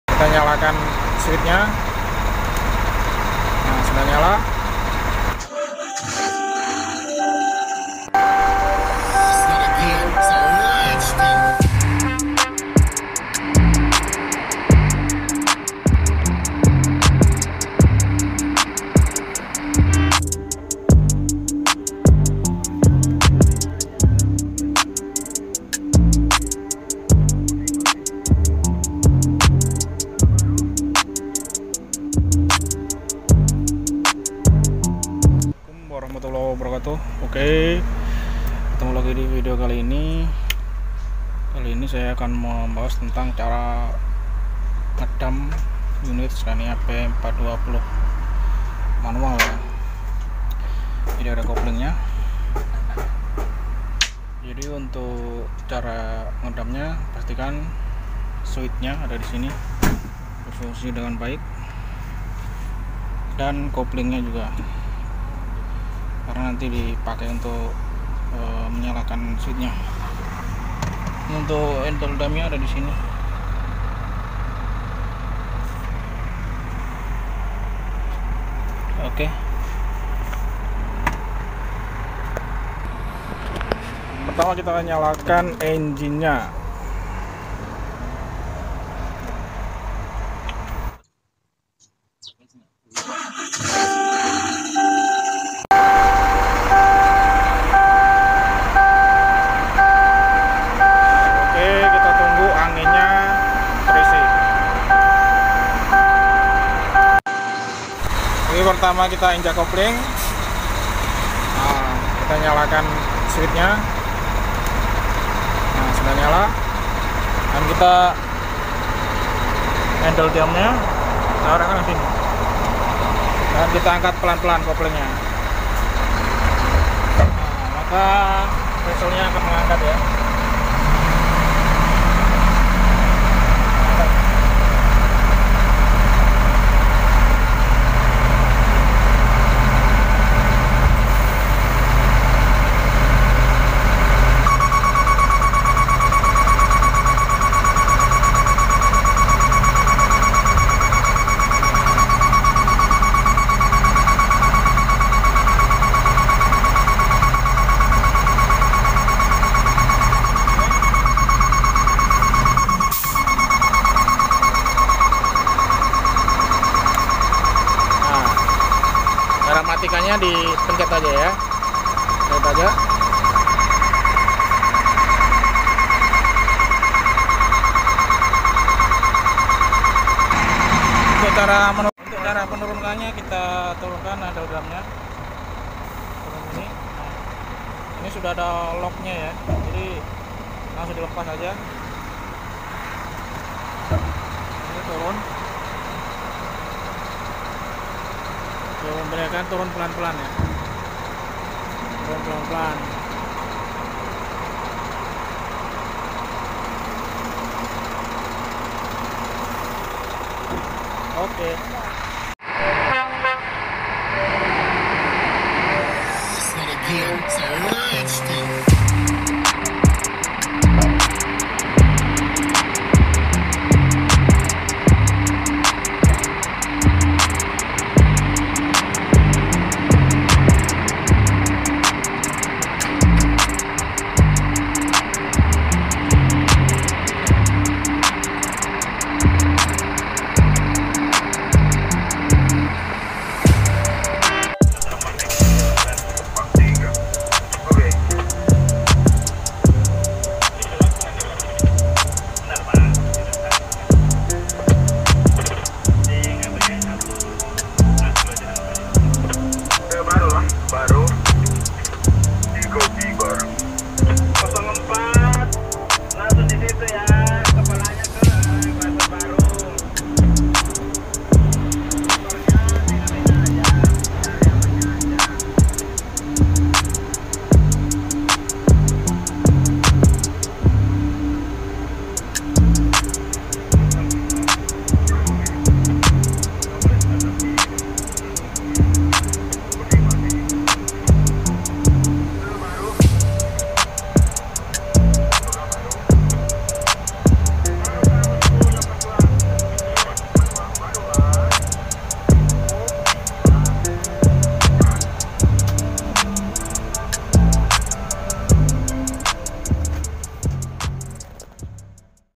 cara ngedump truck scania
pertama pastikan transmisi dalam kondisi netral,parking brake terpasang, nyalakan engine injak kopling nyalakan swicth PTO dan tarik handle dump.
tekanan angin 6-10 bar
dan pada saat P2H pastikan oil hydrolic level dalam kondisi aman serta tidak ada kebocoran hose hydrolic

semoga bermanfaat dan safety first